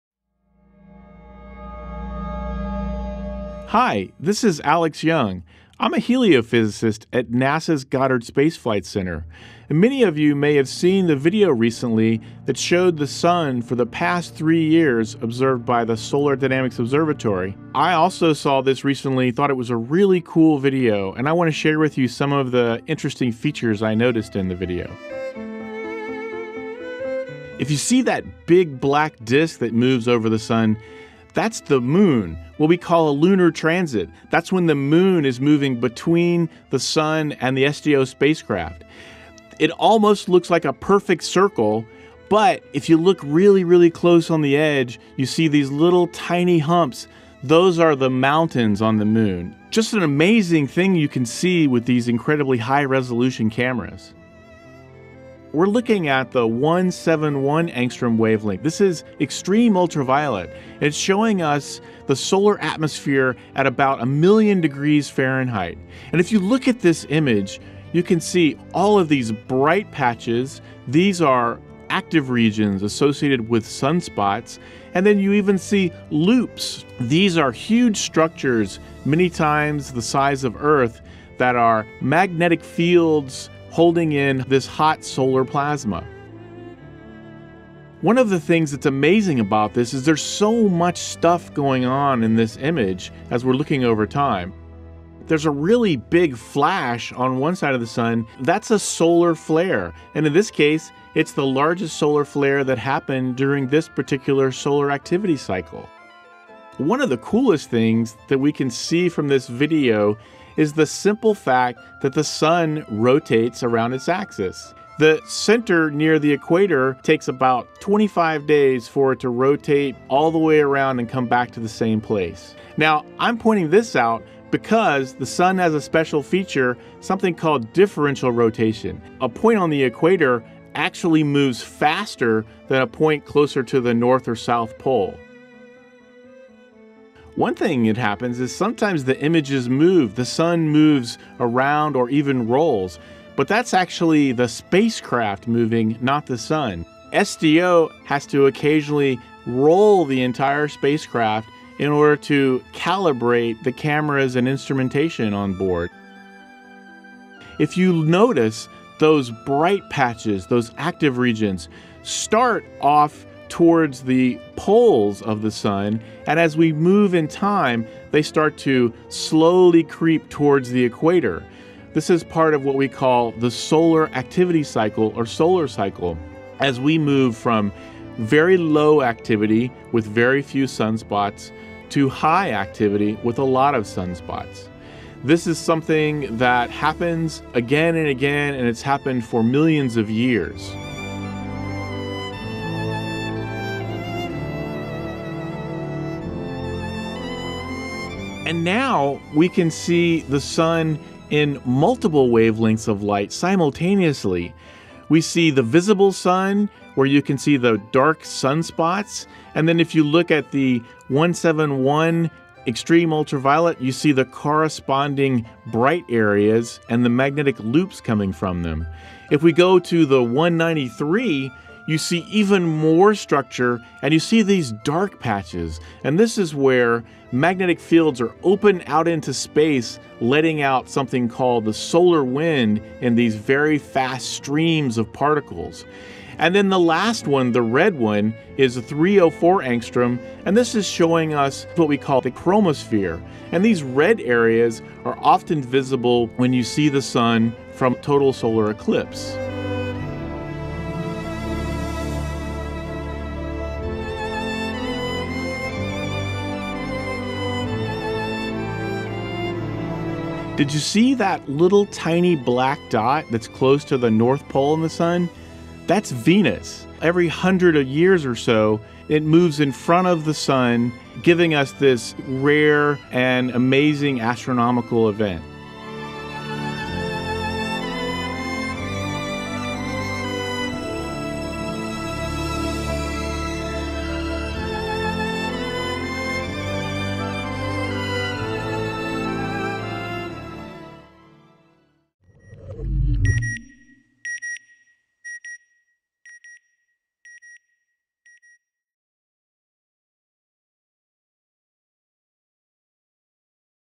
Three Years in Three Minutes With Expert Commentary
Explore three years of mesmerizing solar activity captured by NASA's Solar Dynamics Observatory (SDO) in just three minutes! 🌞☀️ Watch in awe as massive solar flares, stunning prominences, and intricate coronal loops dance across the sun's surface. This captivating compilation showcases the raw power and beauty of our closest star. Join us on this visual journey through the sun's dynamic and ever-changing behavior.